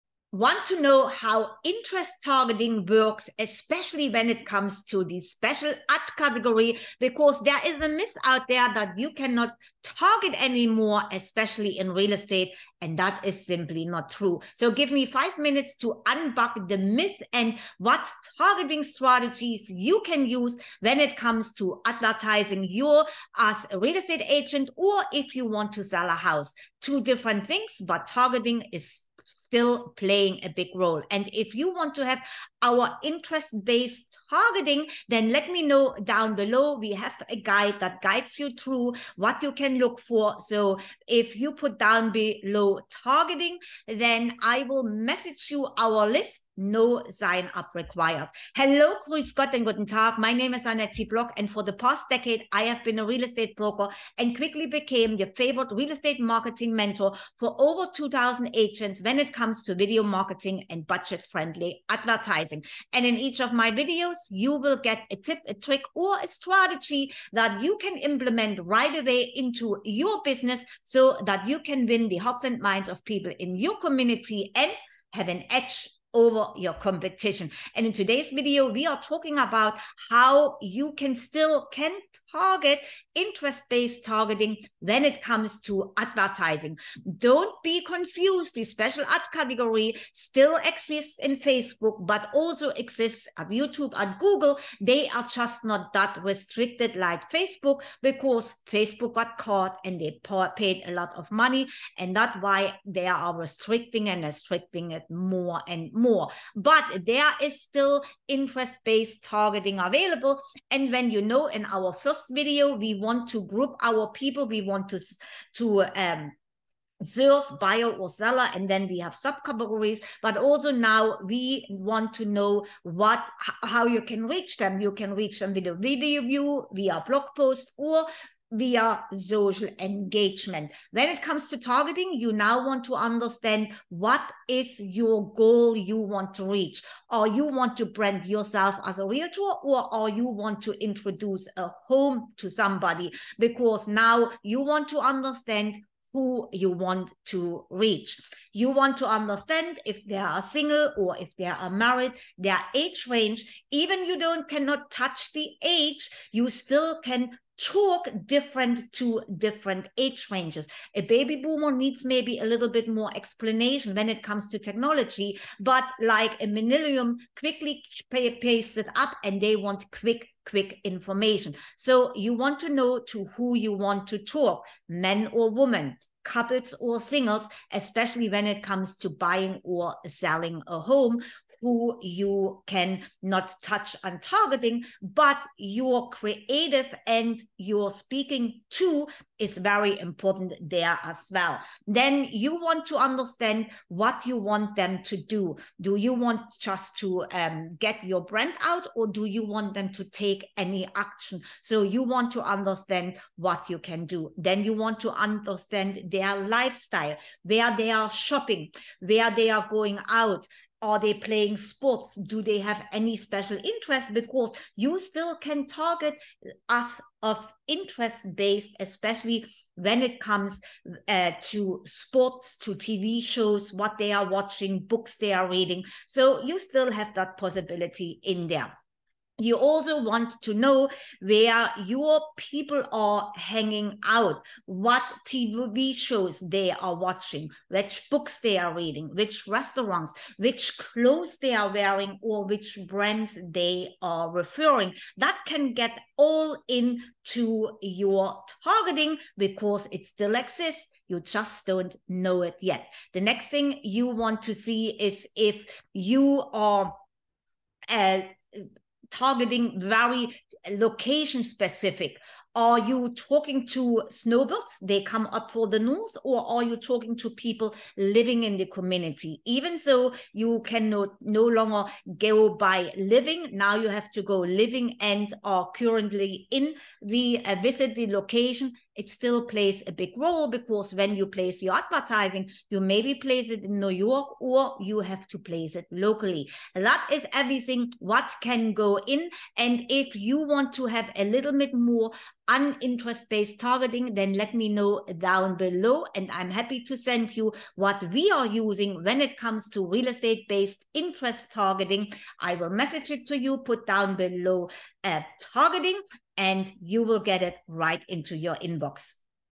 How Real Estate Ads on Facebook work?
Want to know how real estate ads on facebook work with interest targeting works for the real estate ads under the special housing category on Facebook? You are in the right place where in the next 5 min I will debug the myth that targeting is no longer available for the real estate industry.

Want our interest based targeting list? Drop down below targeting and I will send it to you.